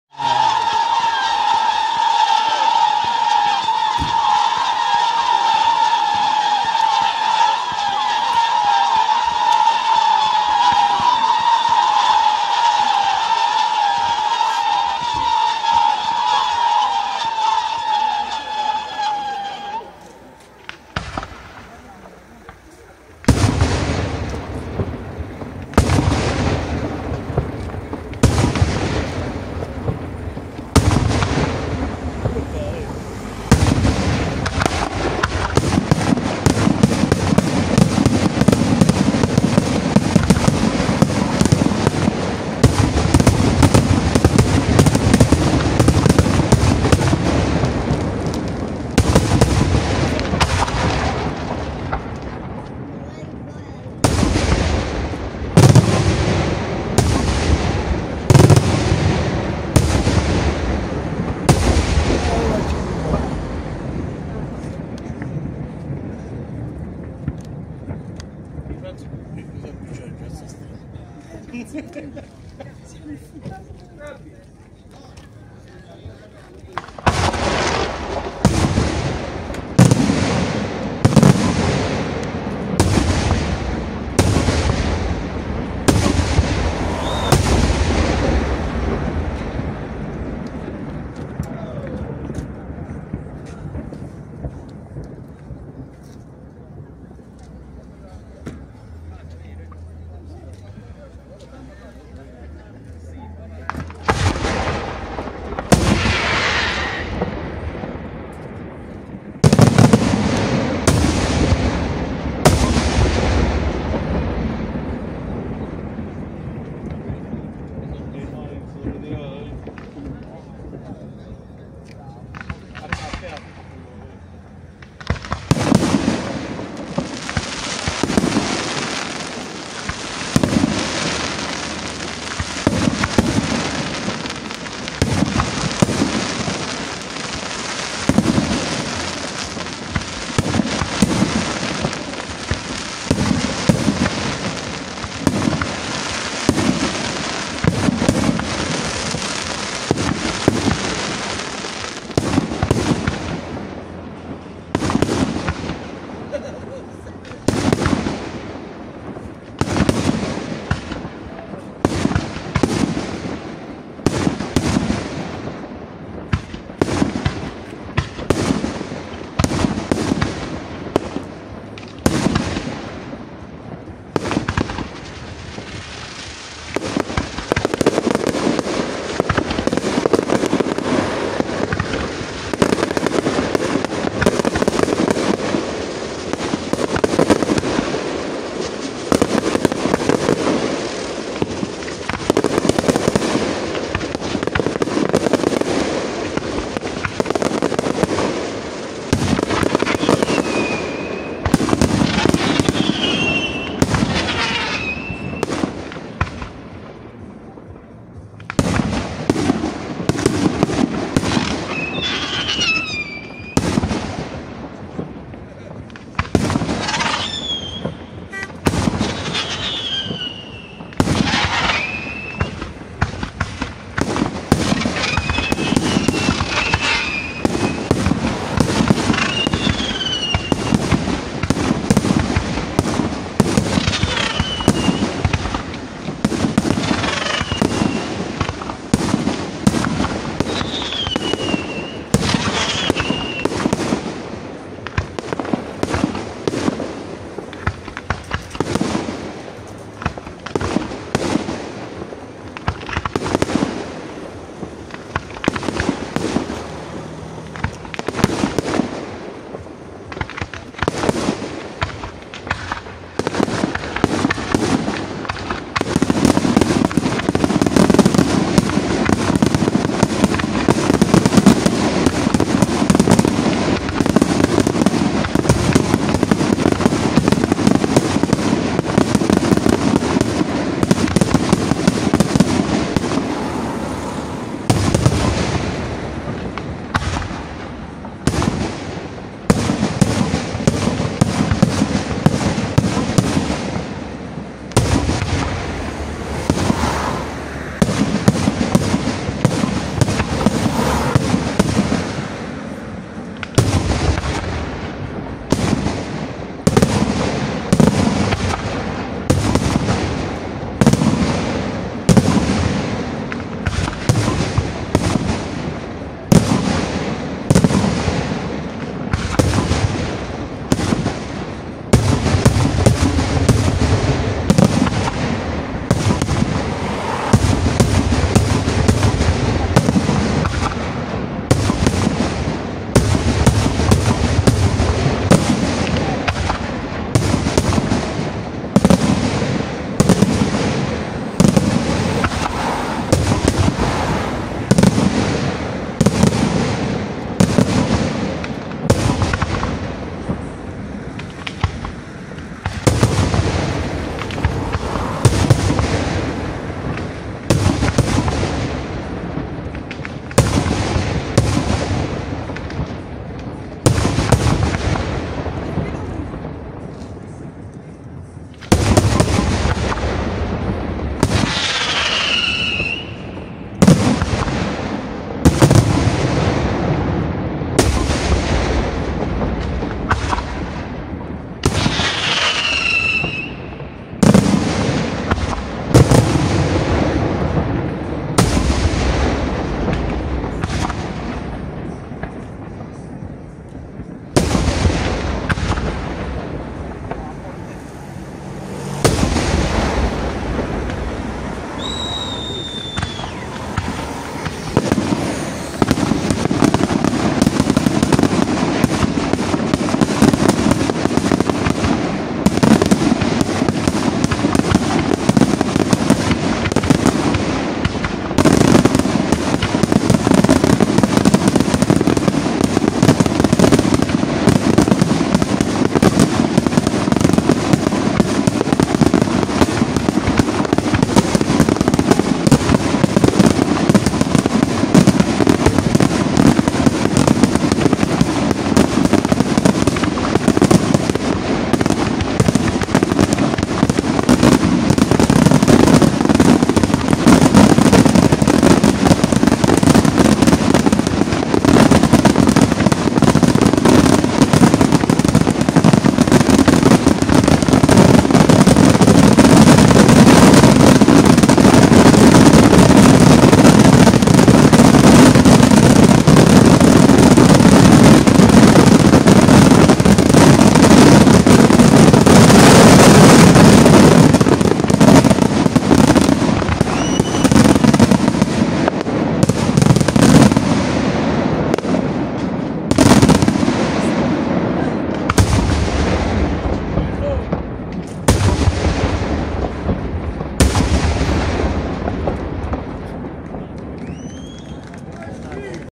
𝐀𝐈𝐑𝐎𝐋𝐀 (𝐁𝐧) - 𝐌𝐀𝐑𝐈𝐀 𝐒𝐬. 𝐀𝐃𝐃𝐎𝐋𝐎𝐑𝐀𝐓𝐀 𝟐𝟎𝟐𝟒 - 𝐏𝐈𝐑𝐎𝐓𝐄𝐂𝐍𝐈𝐂𝐀 𝐑𝐔𝐒𝐒𝐎 𝐞 𝐀𝐋𝐁𝐀𝐍𝐎 (𝐍𝐢𝐠𝐡𝐭 𝐒𝐡𝐨𝐰)
𝐀𝐢𝐫𝐨𝐥𝐚 (𝐁𝐧) - 𝟏𝟕 𝐒𝐞𝐭𝐭𝐞𝐦𝐛𝐫𝐞 𝟐𝟎𝟐𝟒
𝐅𝐞𝐬𝐭𝐞𝐠𝐠𝐢𝐚𝐦𝐞𝐧𝐭𝐢 𝐢𝐧 𝐨𝐧𝐨𝐫𝐞 𝐝𝐢 𝐌𝐚𝐫𝐢𝐚 𝐒𝐬. 𝐀𝐝𝐝𝐨𝐥𝐨𝐫𝐚𝐭𝐚
𝐆𝐚𝐫𝐚 𝐏𝐢𝐫𝐨𝐭𝐞𝐜𝐧𝐢𝐜𝐚 𝐍𝐨𝐭𝐭𝐮𝐫𝐧𝐨
𝐏𝐈𝐑𝐎𝐓𝐄𝐂𝐍𝐈𝐂𝐀 𝐑𝐔𝐒𝐒𝐎 𝐞 𝐀𝐋𝐁𝐀𝐍𝐎 𝐝𝐚 𝐌𝐞𝐥𝐢𝐭𝐨 (𝐍𝐚)